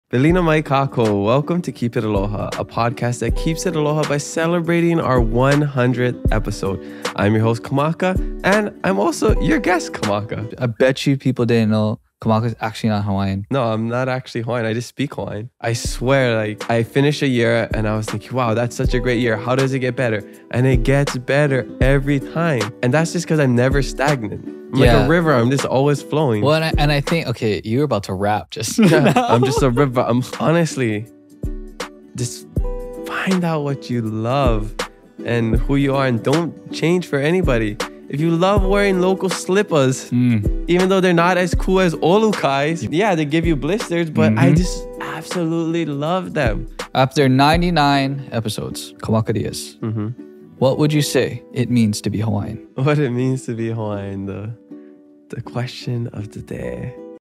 Episode 100 trailer: Kamaka Dias 🤗
Coming up next on Keep it Aloha: The 100th episode 👀

Talk stories with host @kamakadias8 and producer/videographer Jordan Tano on the K.I.A. podcast 🤙🏽

In this episode we talk about Kamaka’s life update, reflect on 99 previous episodes, lessons from podcasting, life in general, and so much more.